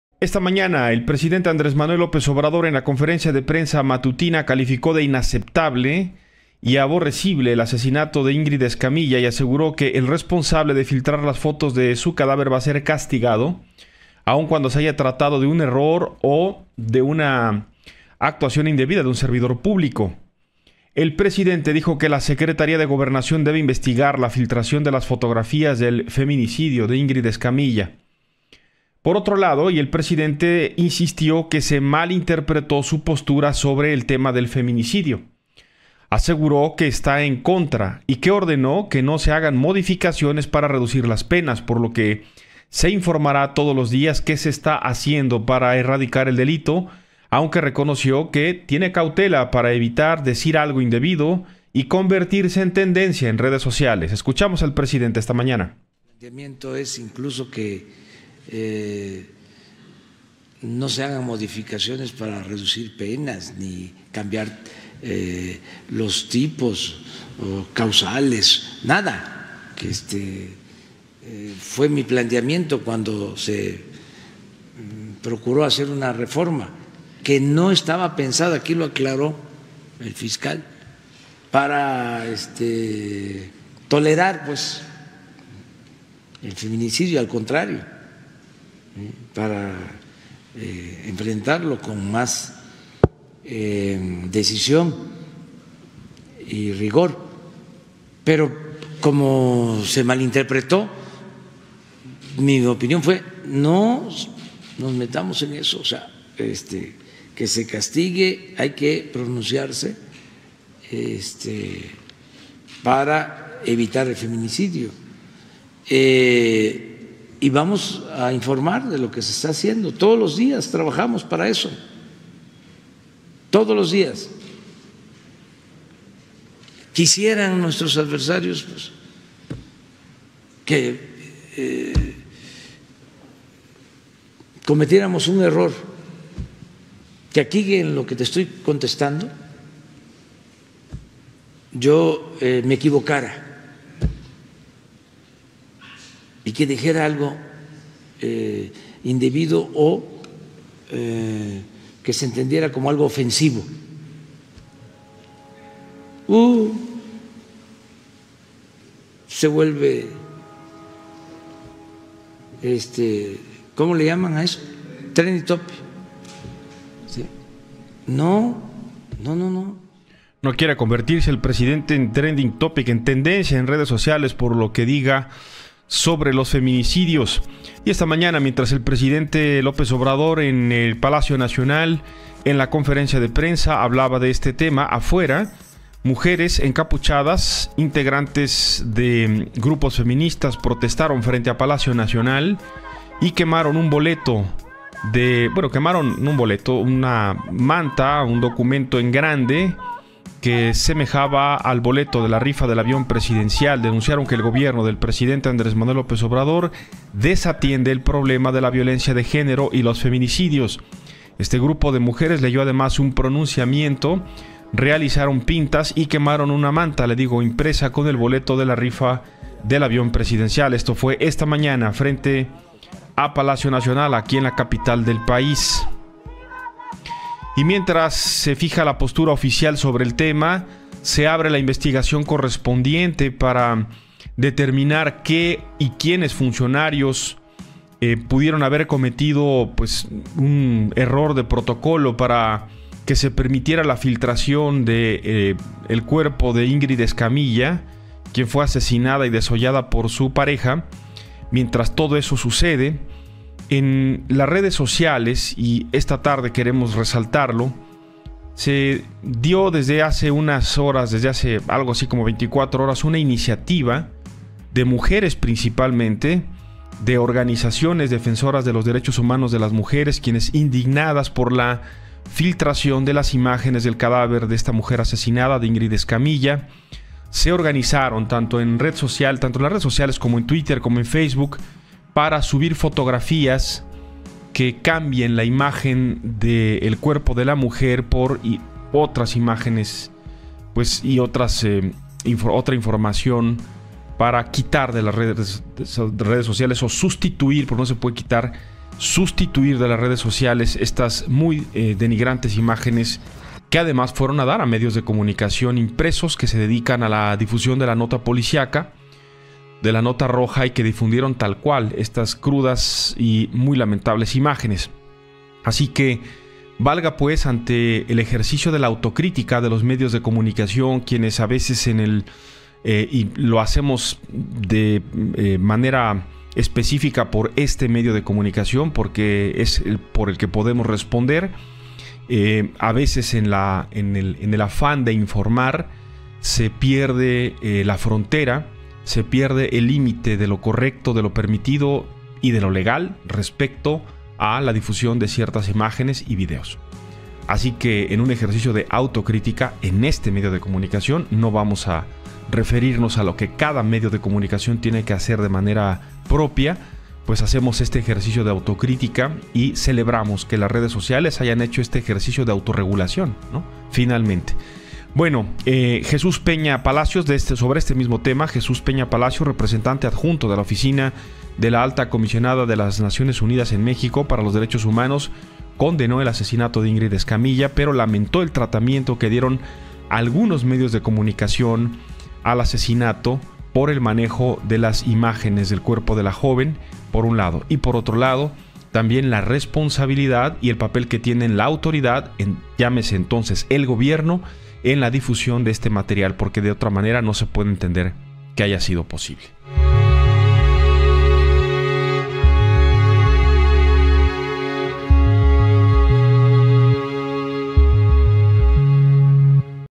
AMLO Reacciona al Asesinato de Ingrid Escamilla
La Secretaría de Gobernación (Segob) ratificó  a través de un comunicado su condena a la publicación y difusión de las imágenes sobre el feminicidio de Ingrid Escamilla Vargas e informó que investigará a los medios de comunicación que las difundieron.